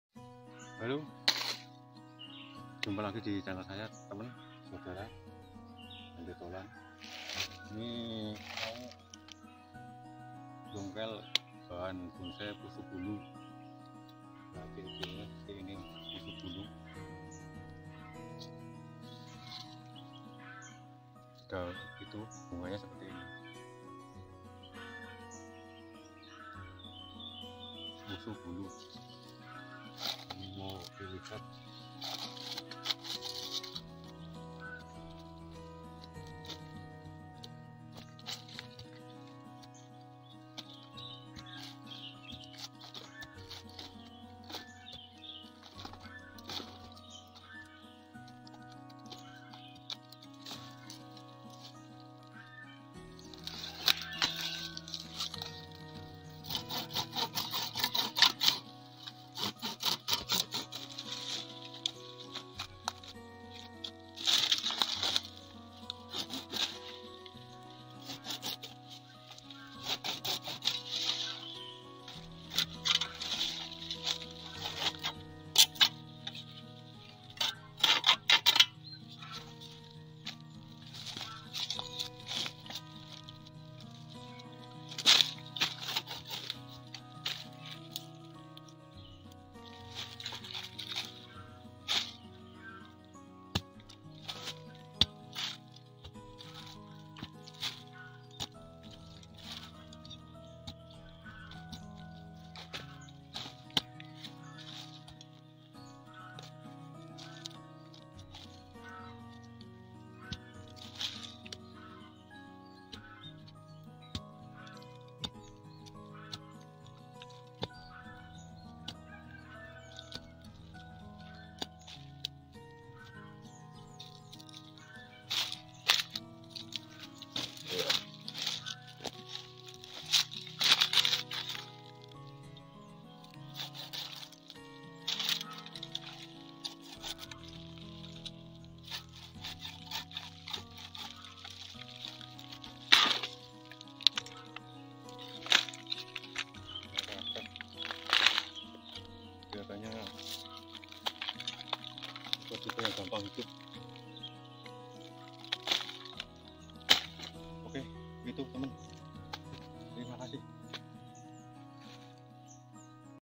ciri ciri dan habitat bahan bonsai pushu bulu di alam liar
jumpa lagi sedulur bonsai..

ini Video tentang ciri dan habitat bahan bonsai phusu bulu.setahu saya seperti ini maaf kalau ada perbedaan di daerah sedulur.